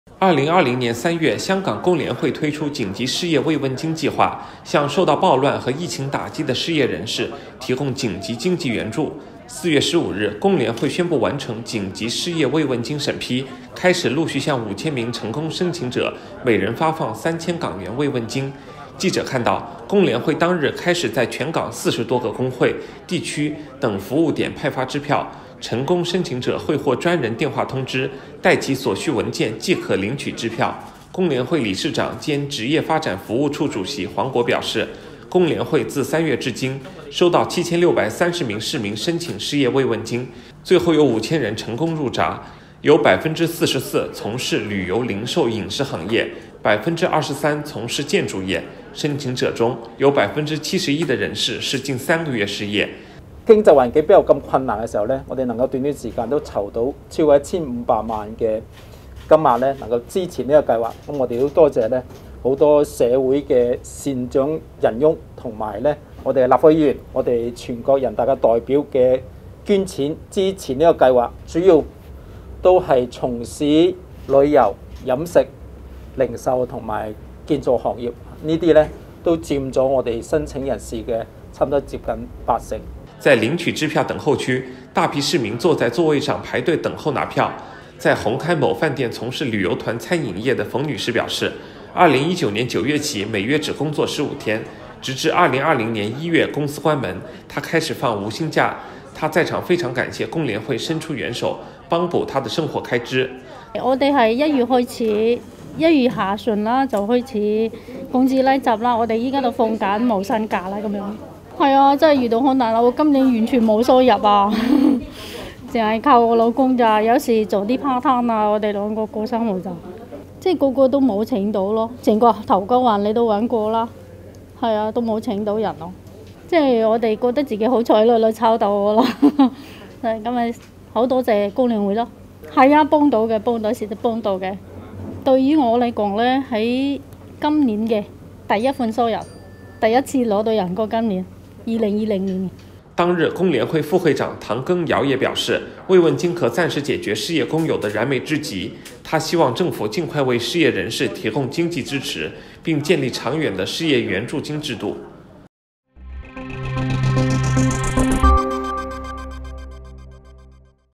香港工聯會緊急失業慰問金今起陸續發放 每人可領3000元
2020年3月香港工聯會推出“緊急失業慰問金計劃”，向受到暴亂和疫情打擊的失業人士提供緊急經濟援助。4月15日起，工聯會宣佈完成“緊急失業慰問金”審批，開始陸續向5000名成功申請者發放3000港元慰問金。記者看到，工聯會當日開始在全港四十多個工會、地區等服務點派發支票，成功申請者會獲專人電話通知，帶齊所需文件即可領取支票。工聯會理事長兼職業發展服務處主席黃國表示，工聯會自3月至今，收到7630名市民申請失業慰問金，最後有5000人成功“入閘”，有44%從事旅遊、零售、飲食行業，23%從事建築業。申請者中，有71%的人士是近三個月失業。